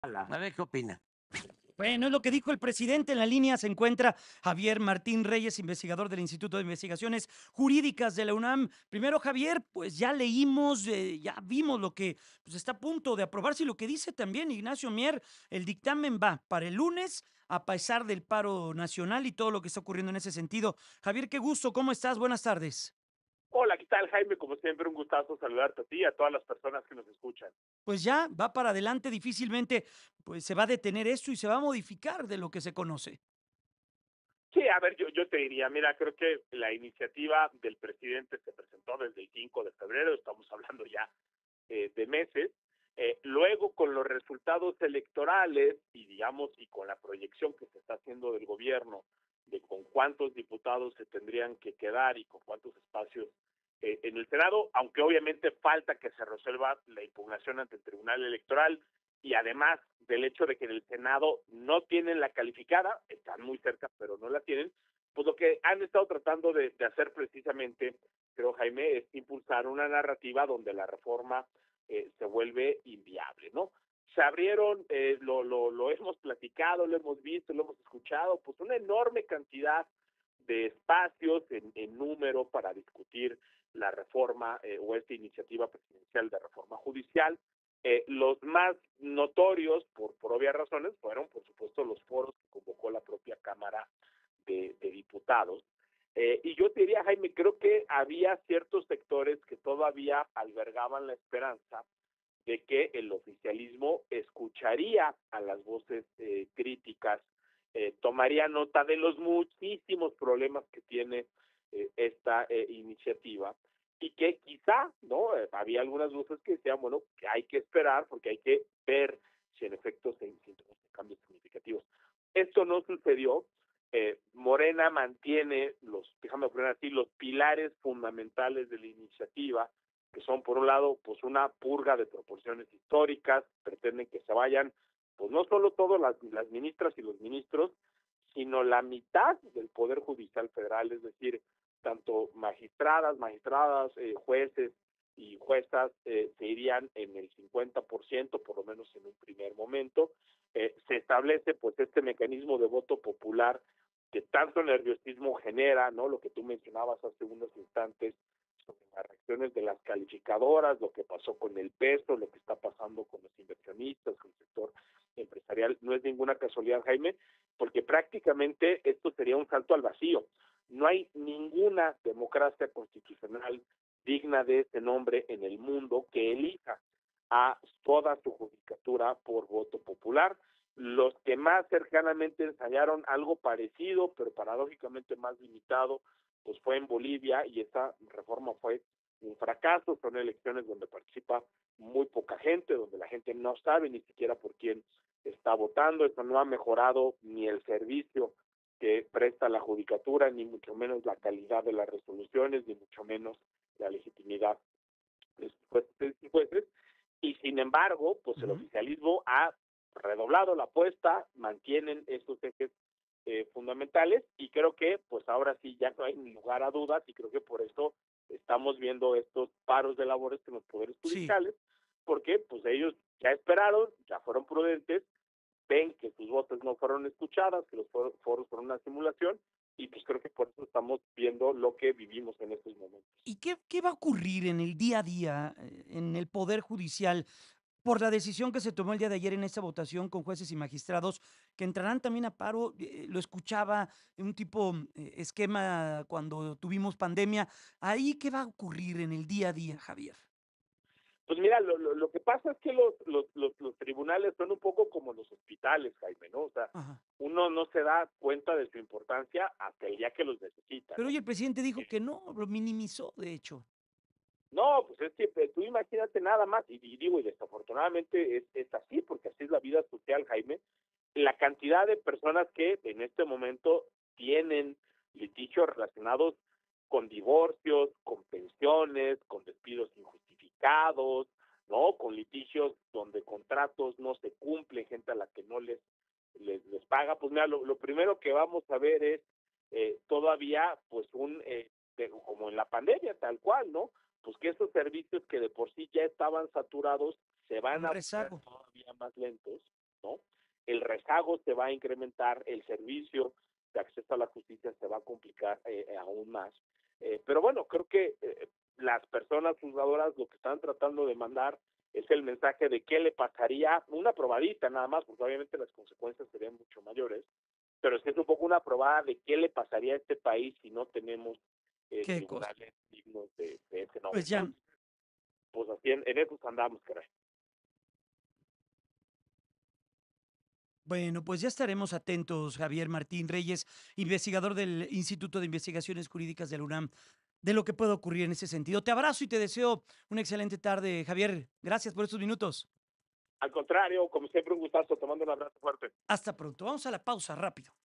Es un 'salto al vacío': Investigador de la UNAM sobre elección de jueces y ministros
Javier Martín Reyes, investigador del Instituto de Investigaciones Jurídicas de la UNAM, abordó la reforma al Poder Judicial que propone elegir jueces, ministros y magistrados mediante voto popular. Según Reyes, esto representaría un riesgo, ya que ninguna democracia en el mundo lo practica, excepto en Bolivia, donde considera que la experiencia fue un fracaso debido a la baja participación ciudadana.
Grupo Fórmula